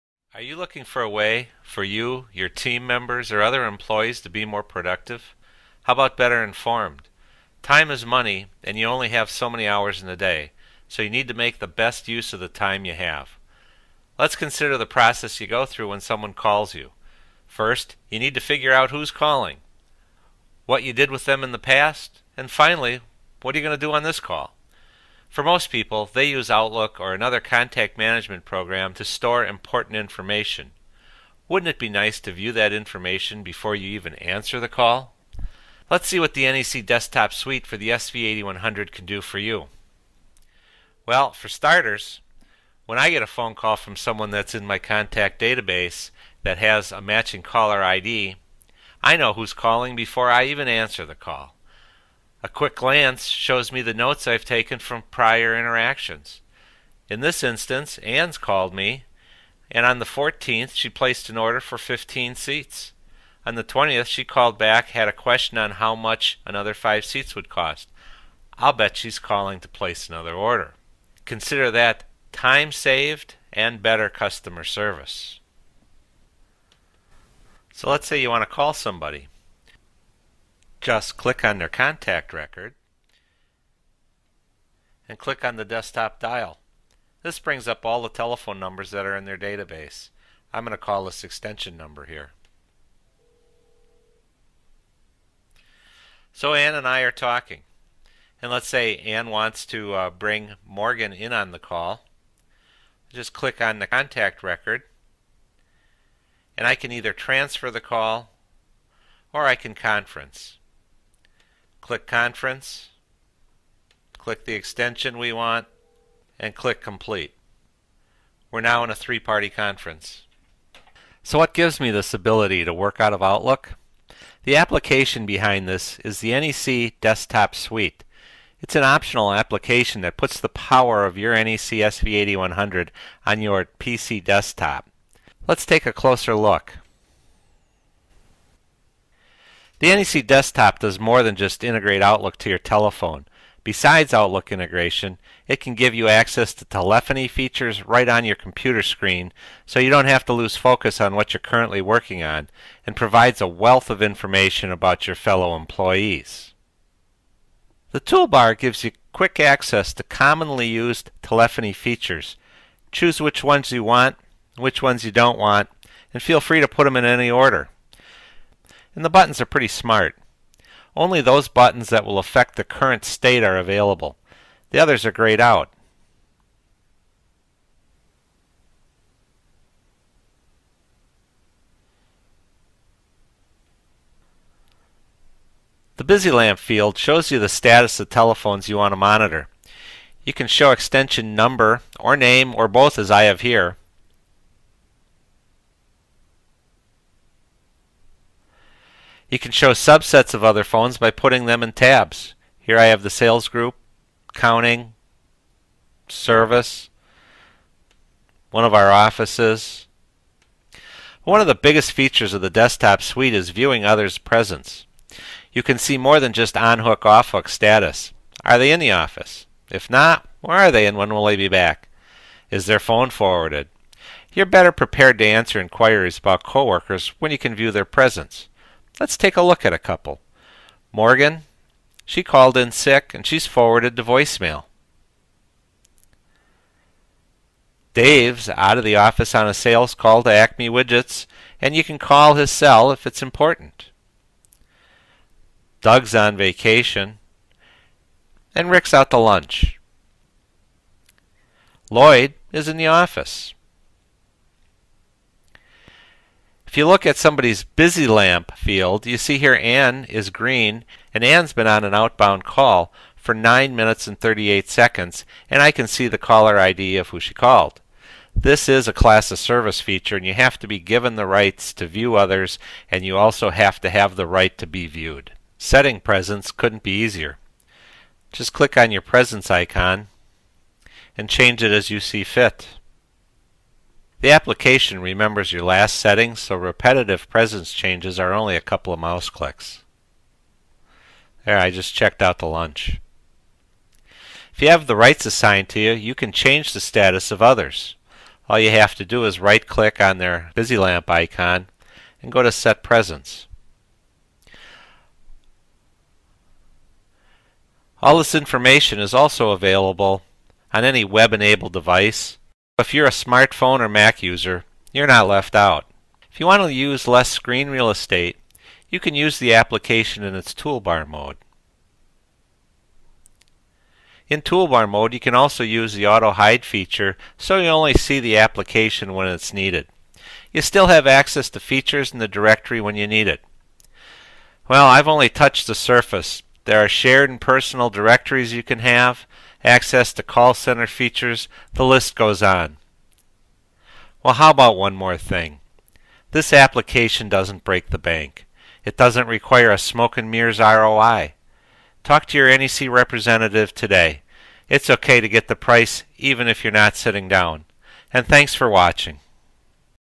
Using NEC Desktop Suite
A general overview of how to use UC Desktop Suite.

If you wish to learn more about Desktop Suite, visit ECI's